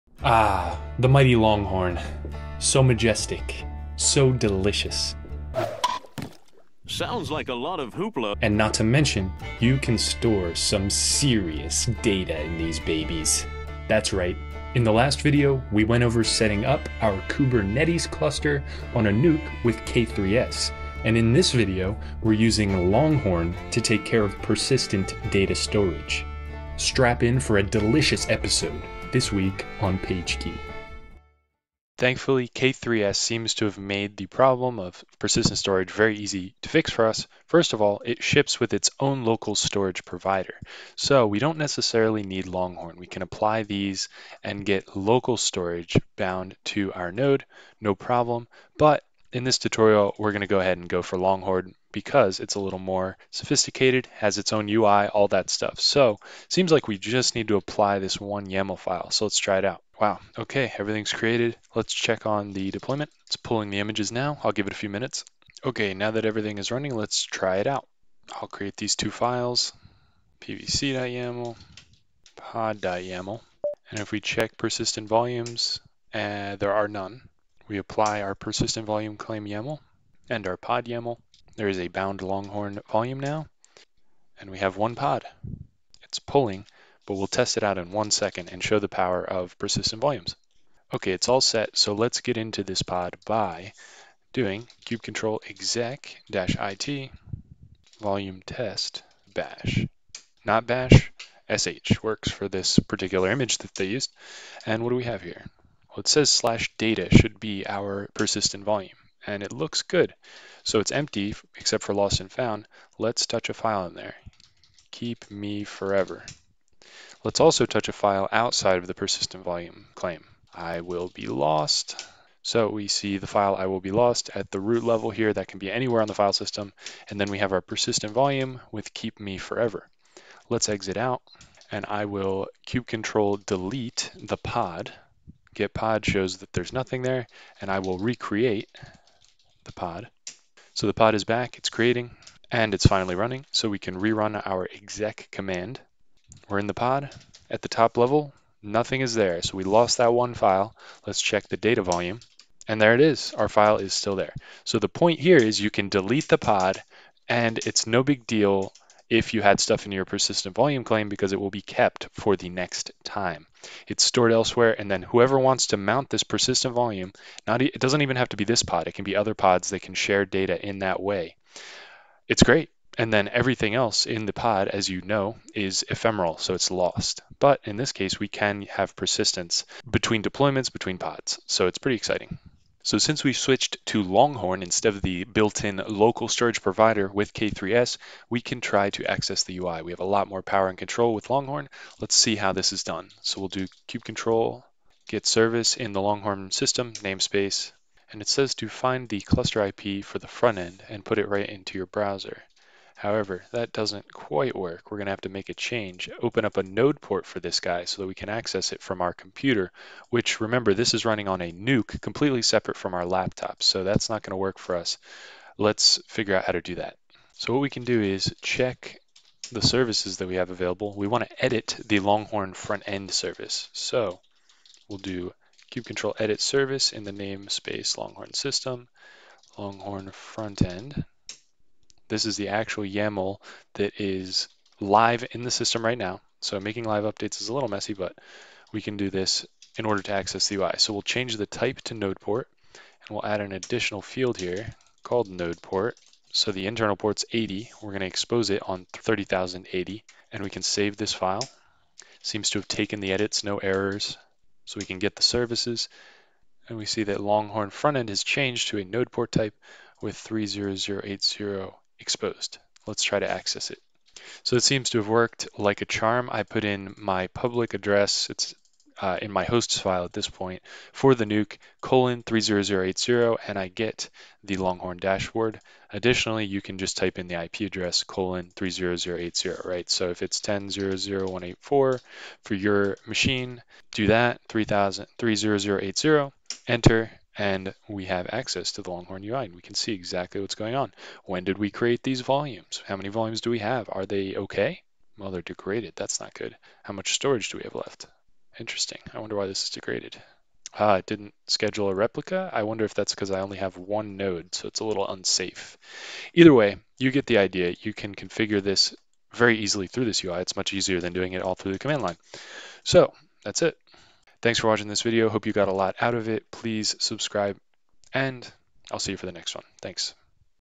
How to: Kubernetes Storage with Longhorn
Let's wrangle us some Longhorn and install it on our cluster.

PageKey Tech is a channel where we take back tech by building our own versions of the products and services we all use every single day.

We can discuss what you want to work on and get you involved!

Timestamps:
0:00 Intro
0:31 Local Storage Provider
0:55 Installing Longhorn
1:05 Usage example
3:02 Longhorn UI